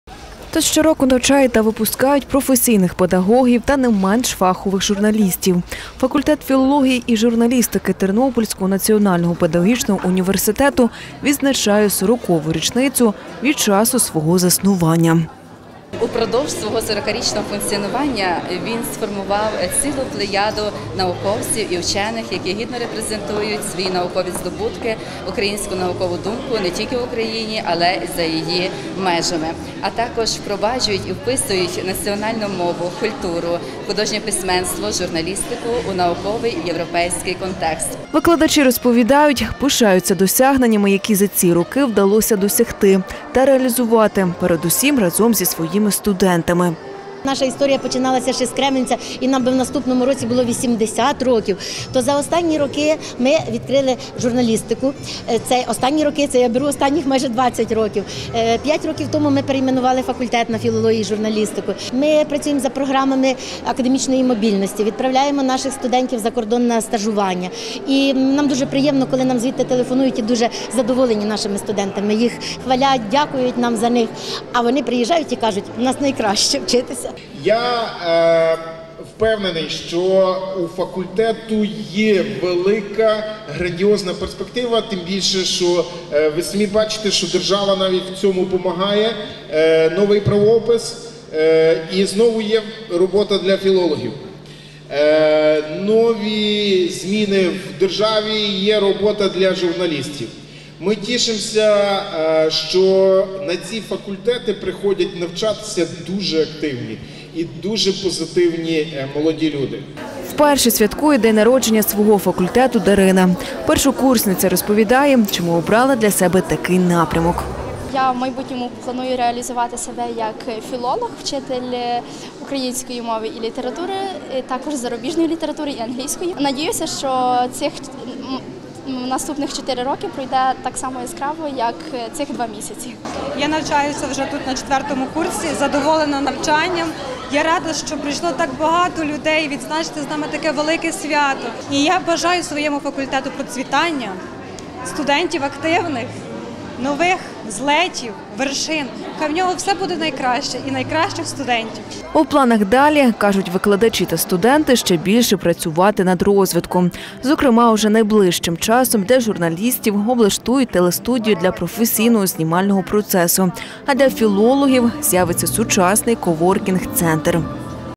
40-річчя святкує факультет філології і журналістики ТНПУ ім. В. Гнатюка
Своє 40-річчя сьогодні святкує факультет філології і журналістики Тернопільського національного педагогічного університету імені Володимира Гнатюка.  Урочиста подія зібрала викладачів та студентів, аби згадати пройдений шлях та щоденні кроки до вдосконалення. Святкову атмофосферу відчули і наші кореспонденти.